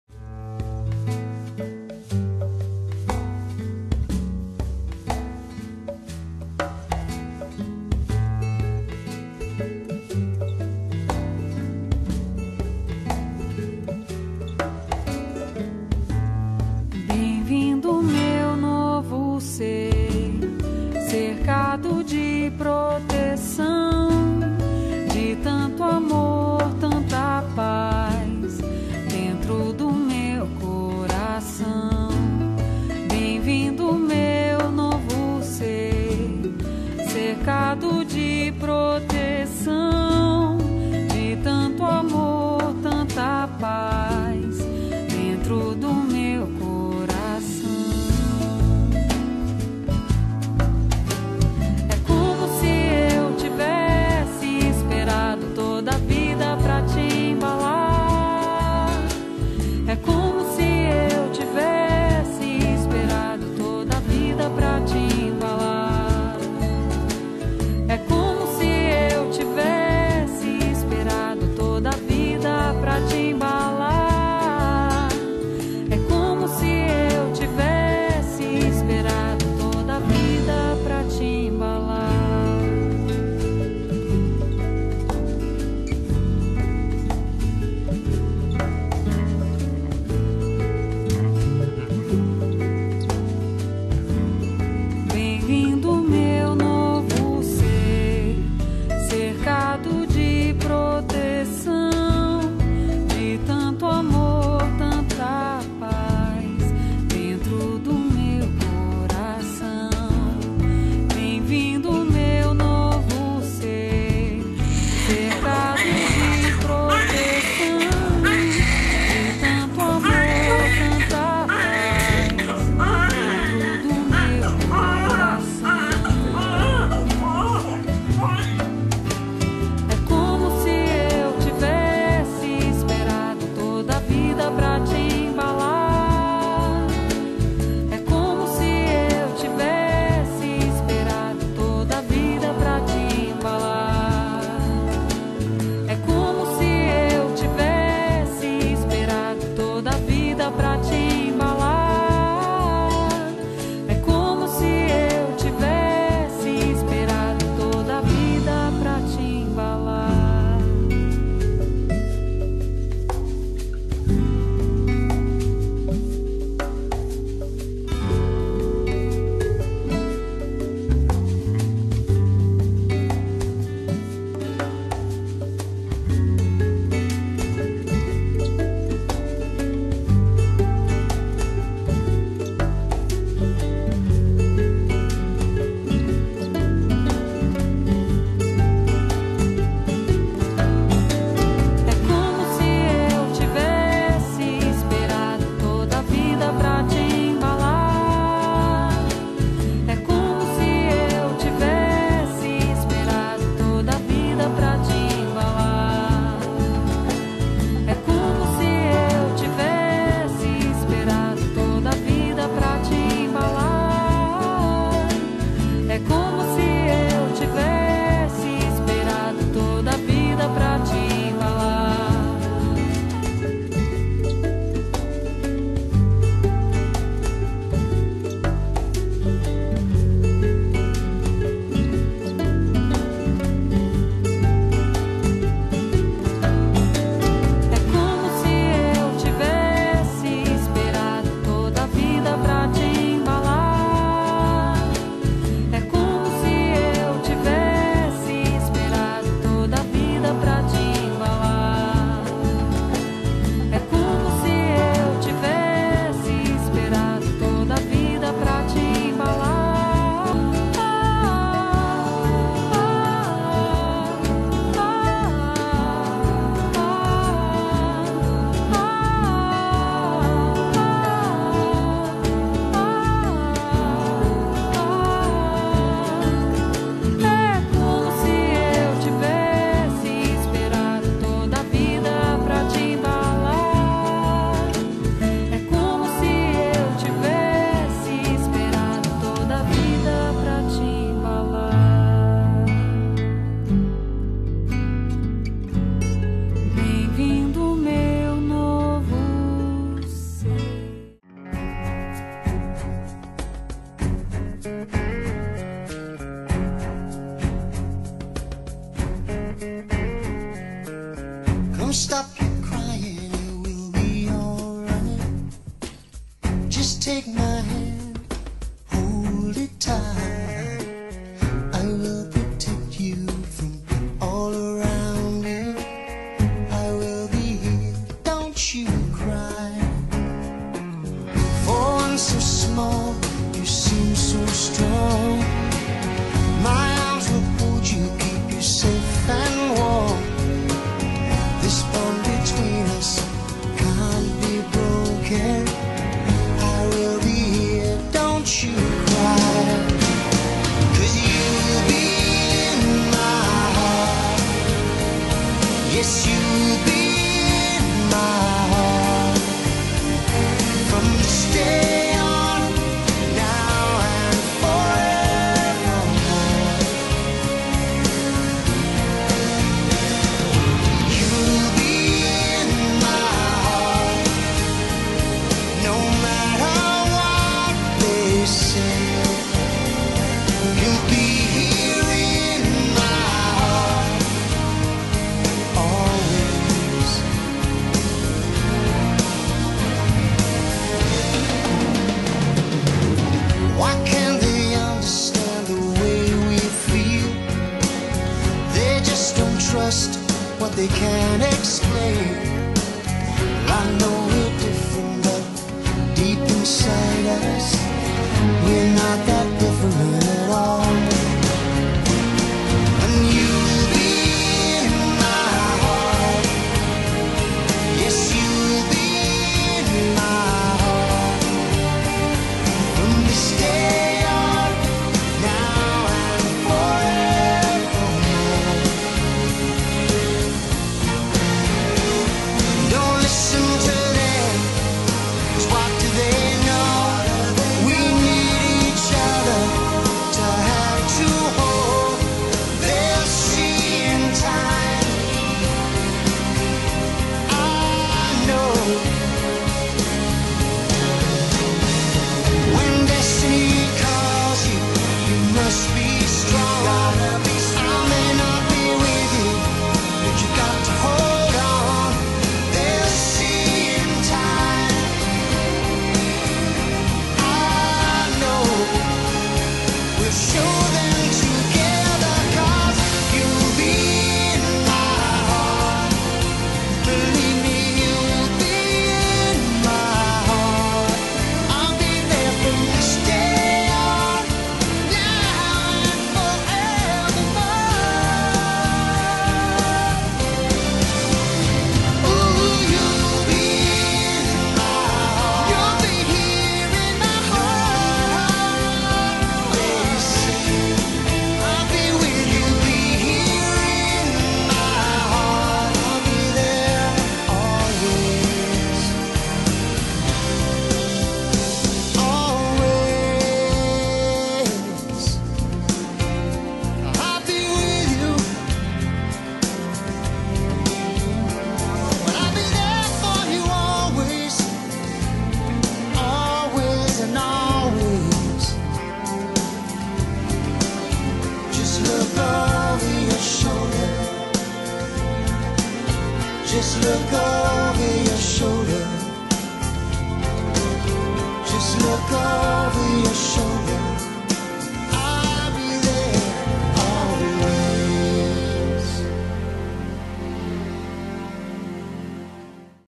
O Nascimento do Rafael
Um lindo parto, cercado de amor e expectativa para a chegada do Rafael. A família inteira comemorou com grande alegria.